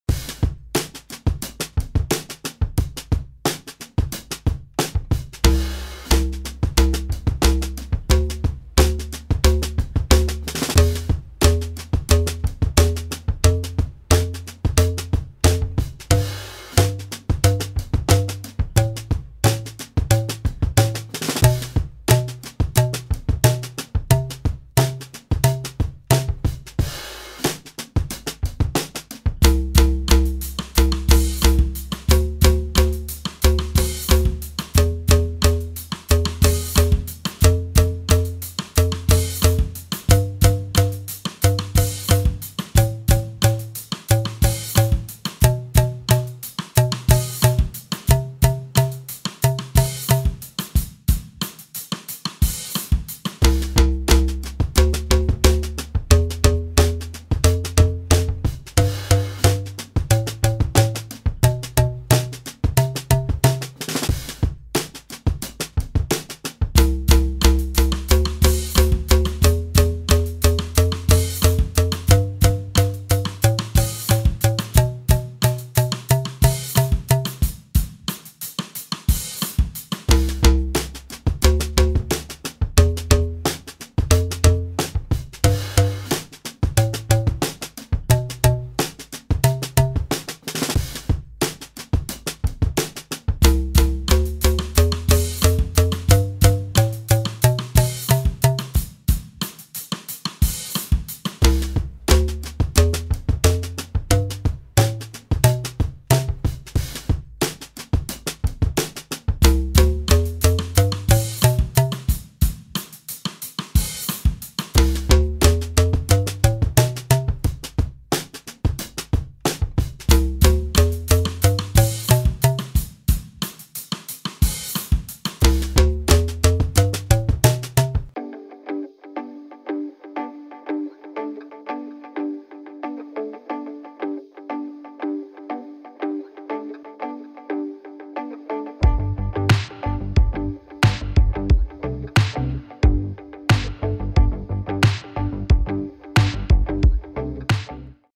2 Minute Boomwhacker Warm-Up | Ascending
This 2 minute Boomwhacker exercise is meant to be the first thing students play at the beginning of your ElementaryMusic Boomwhacker lesson. Take advantage of changing the playback speed if you would like the video to go slower or faster.